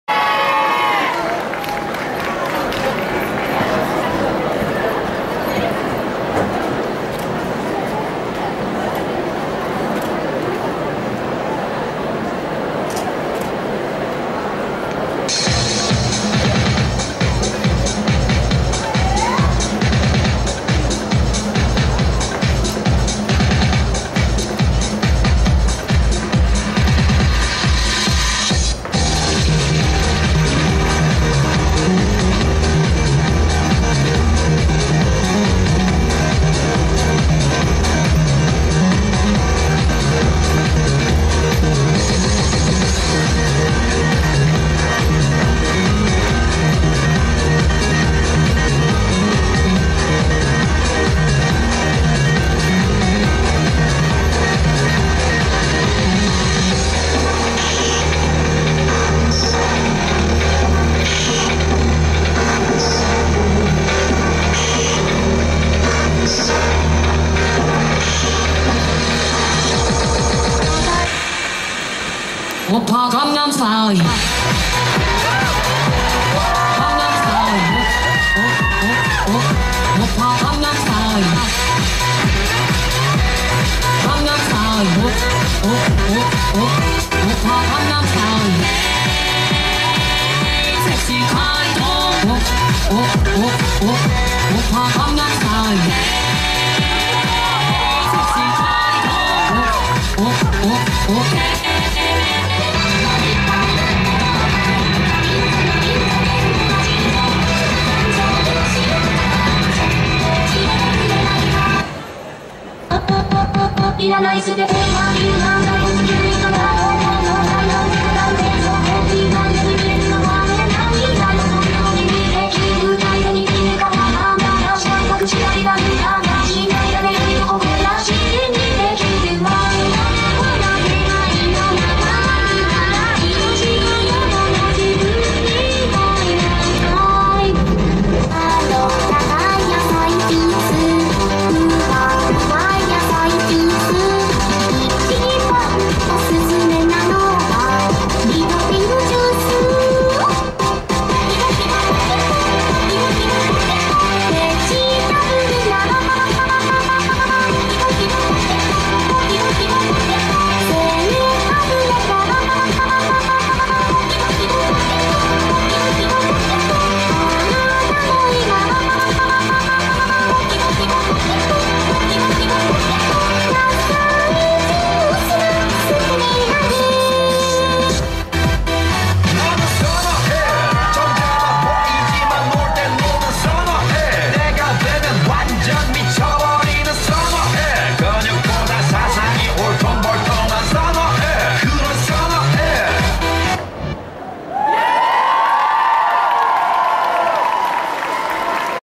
Concours Cosplay du Samedi à Japan Touch 15
Le concours cosplay de Japan Touch 15 était géré par l'association Aoi Sora Cosplay
Cette prestation a été filmée par Kibotori Senpai de l'association Ota'Sekai